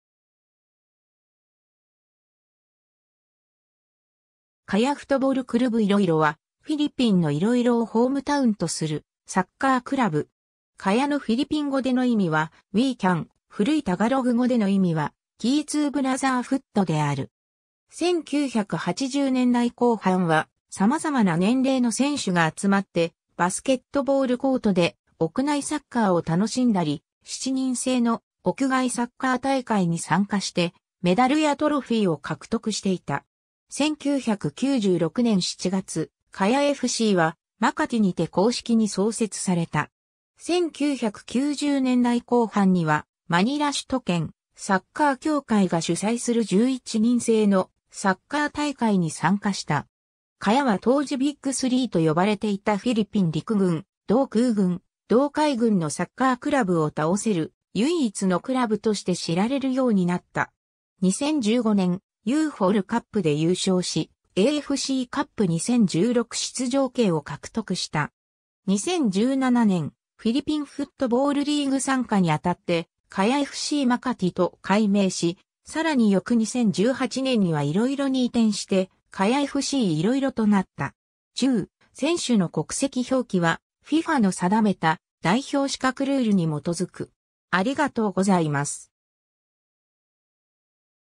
カヤFC
カヤ・フトボル・クルブ-イロイロ (フィリピノ語: Kaya Futbol Club–Iloilo) は、フィリピンのイロイロをホームタウンとするサッカークラブ。
カヤのフィリピン語での意味はWe can、古いタガログ語での意味はkey to brotherhoodである。
 1980年代後半は様々な年齢の選手が集まってバスケットボールコートで屋内サッカーを楽しんだり、7人制の屋外サッカー大会に参加してメダルやトロフィーを獲得していた。
1996年7月、カヤFCはマカティにて公式に創設された。
1990年代後半にはマニラ首都圏サッカー協会が主催する11人制のサッカー大会に参加した。
カヤは当時ビッグスリーと呼ばれていたフィリピン陸軍、同空軍、同海軍のサッカークラブを倒せる唯一のクラブとして知られるようになった。
 2015年、UFLカップで優勝し、AFCカップ2016出場権を獲得した。
 2017年、フィリピン・フットボールリーグ参加にあたって「カヤFC-マカティ」と改名し、さらに翌2018年にはイロイロに移転して「カヤFC-イロイロ」となった。